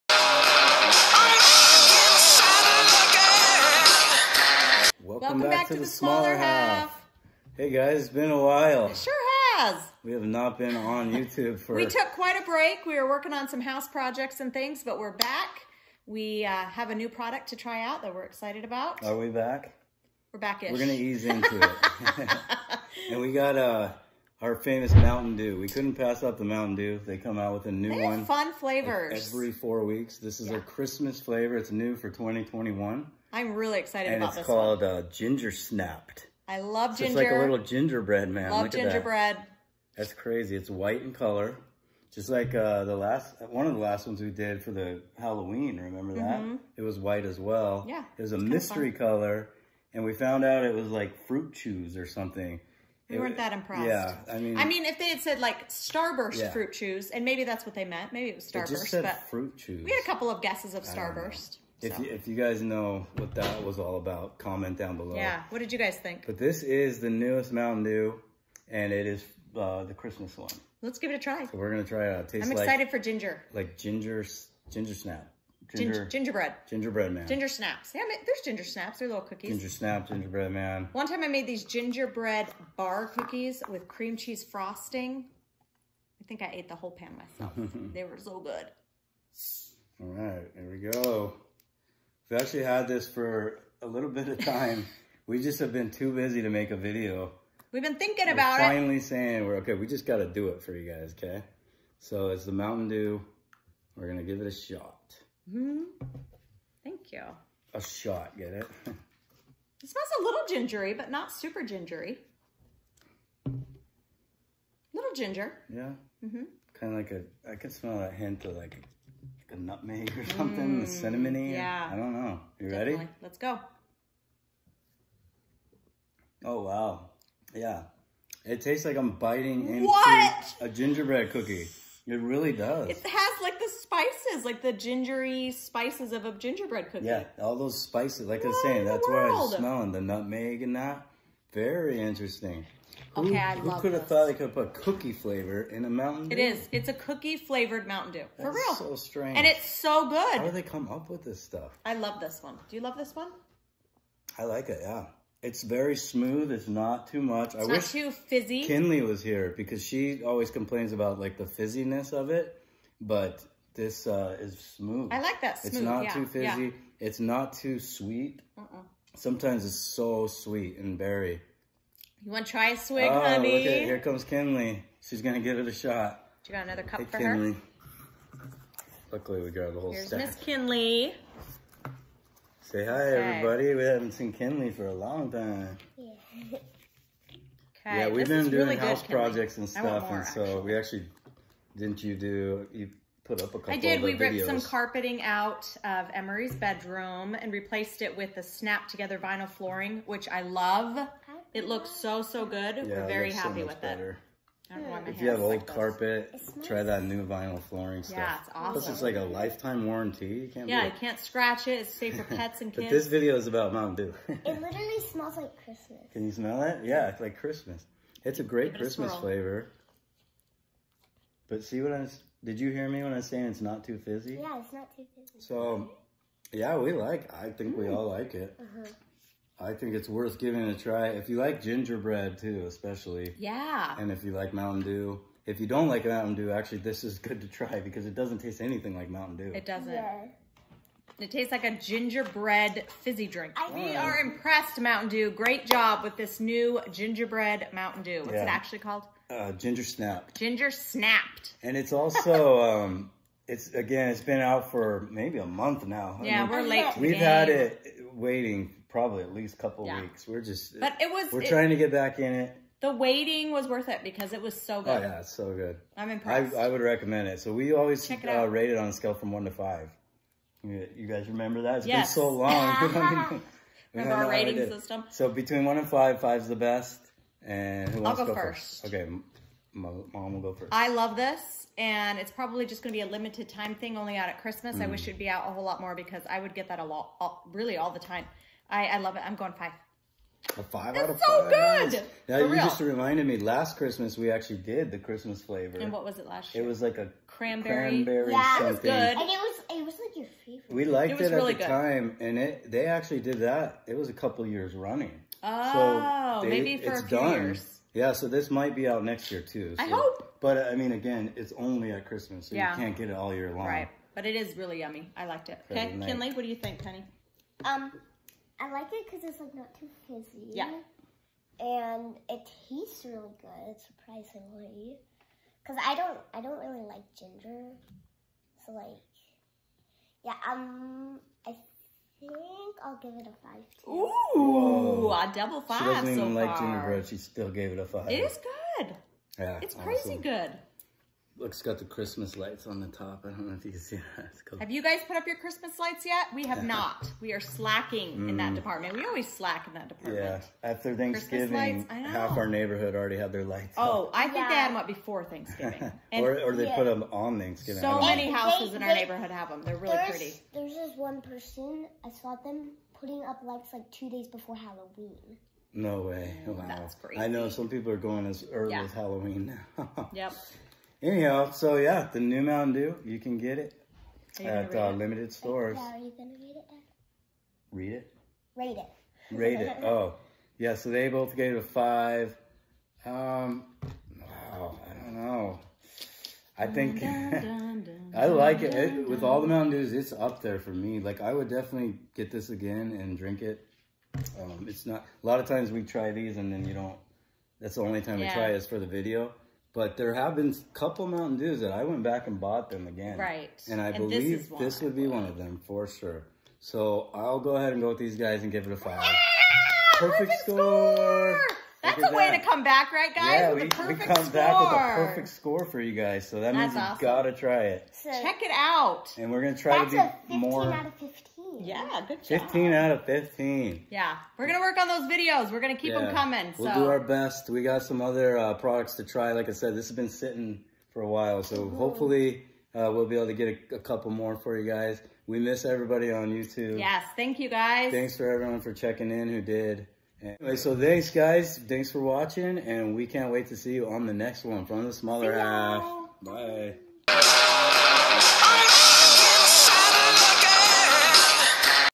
Mtn Dew Gingerbread SNAP’D 2021 Holiday Christmas Flavor
We had to try this new Mountain Dew Flavor Gingerbread Cookies and tell you what we thought.